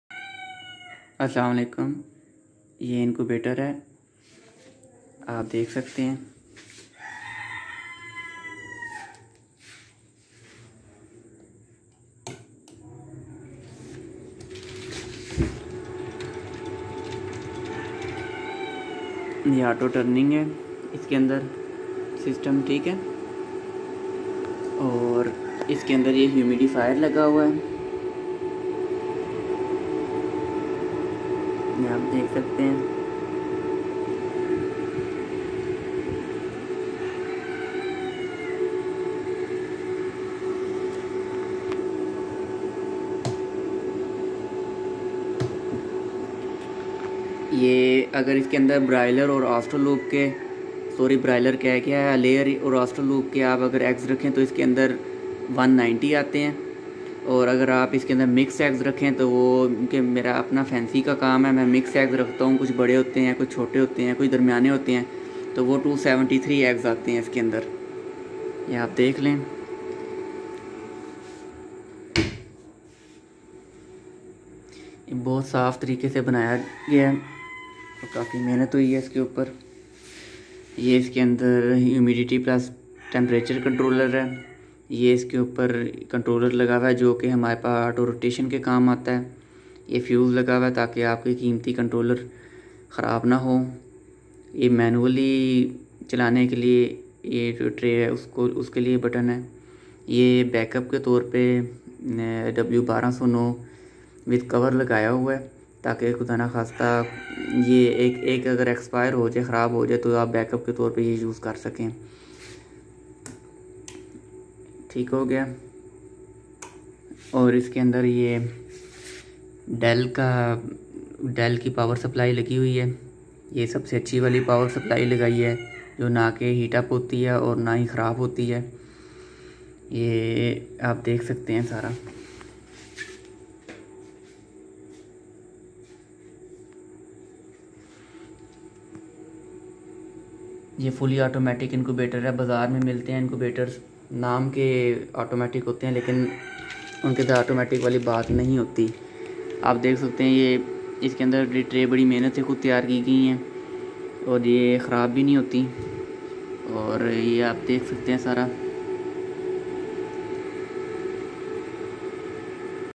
Incubator in pakistan by fmbudgies
Hello friends is video ma AJ apko incubator ka batay jayga ka wo  or is bhai ka number bi diya jayga